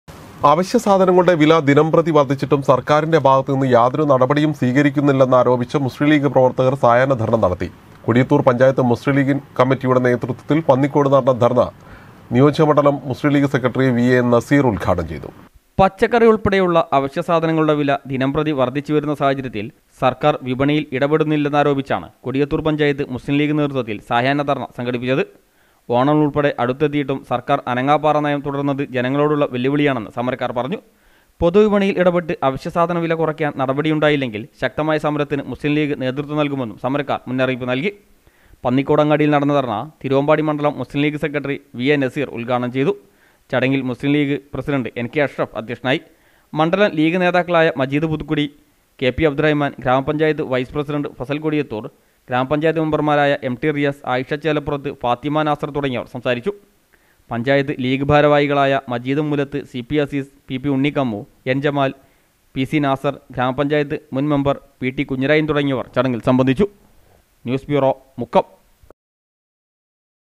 അവശ്യ സാധനങ്ങളുടെ വില ദിനംപ്രതി വർധിച്ചിട്ടും സർക്കാരിൻ്റെ ഭാഗത്ത് നിന്ന് യാതൊരു നടപടിയും
അവശ്യ സാധനങ്ങളുടെ വില ദിനംപ്രതി വർധിച്ചിട്ടും സർക്കാരിൻ്റെ ഭാഗത്ത് നിന്ന് യാതൊരു നടപടിയും സ്വീകരിക്കുന്നില്ലന്നാരോപിച്ച് മുസ്ലീം ലീഗ് പ്രവർത്തകർ സായാഹ്ന ധർണ്ണ നടത്തി.